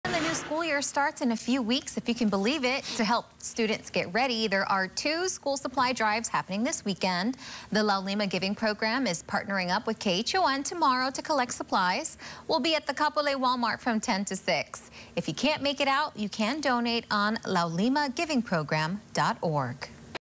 Laulima Giving Program supports keiki in summer back-to-school supply drive
Parents gearing up for the upcoming school year can find a helping hand on July 13 and 27, at Walmart Kapolei.